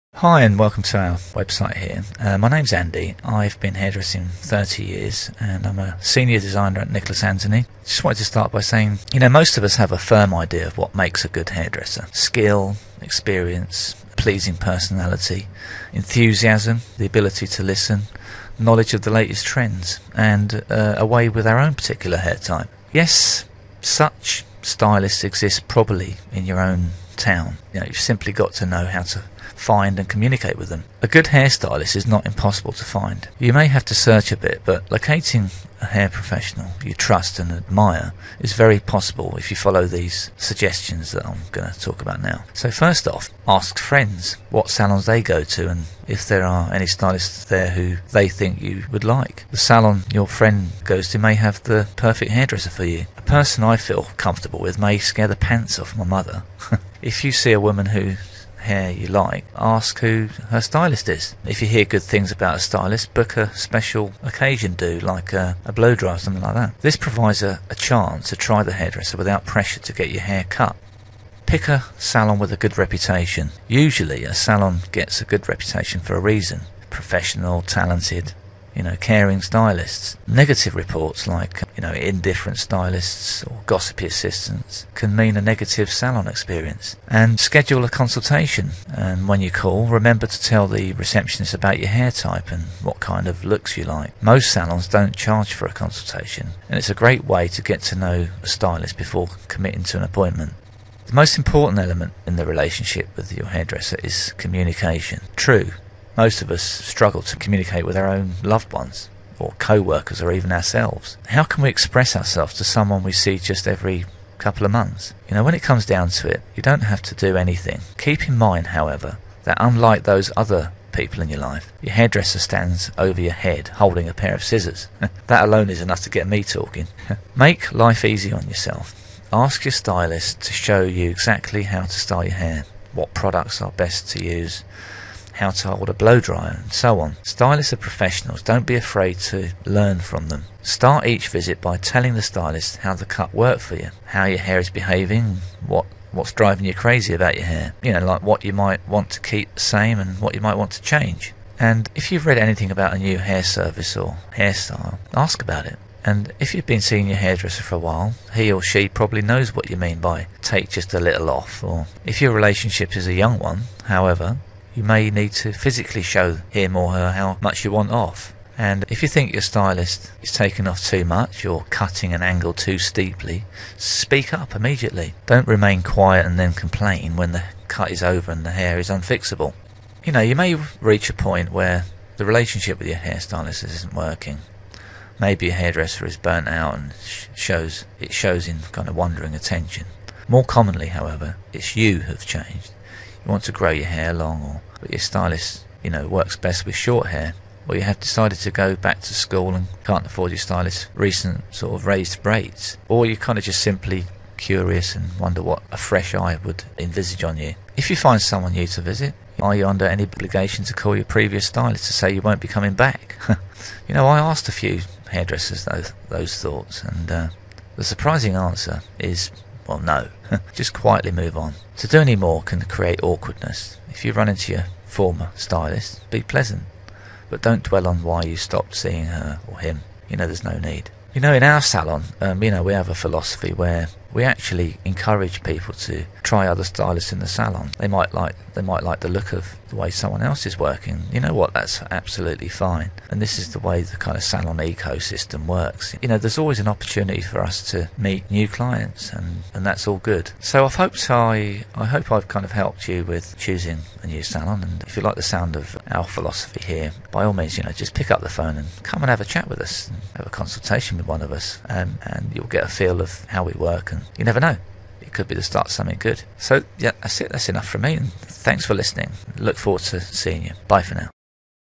Nicholas Anthony Hairdressers
Introduction to Nicholas Anthony. If your looking for a new hairdresser, check this out.